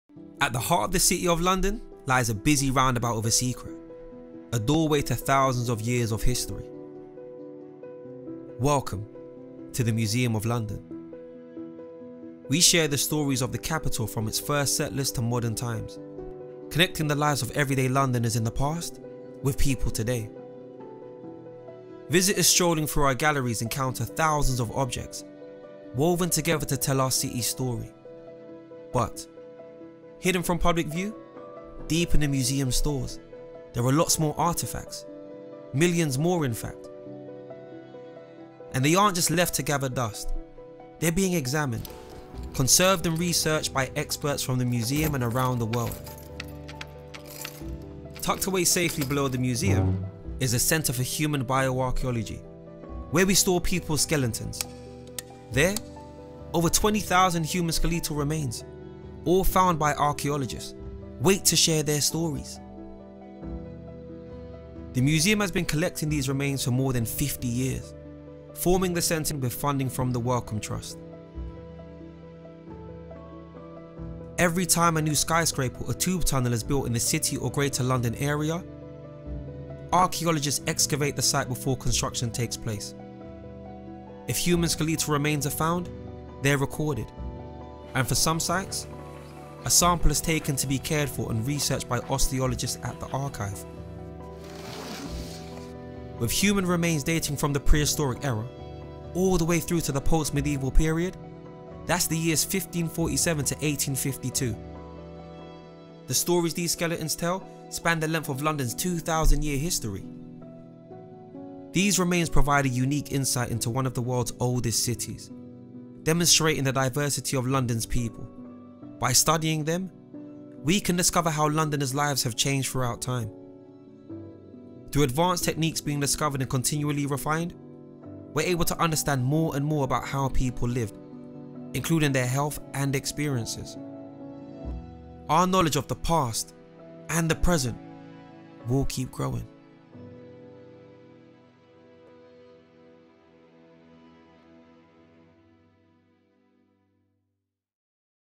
Exploring the Centre for Human Bioarchaeology
This roundabout has a secret! 20,000 human skeletal remains lie hidden below. Get a glimpse inside the Museum of London’s Centre for Human Bioarchaeology in this short film.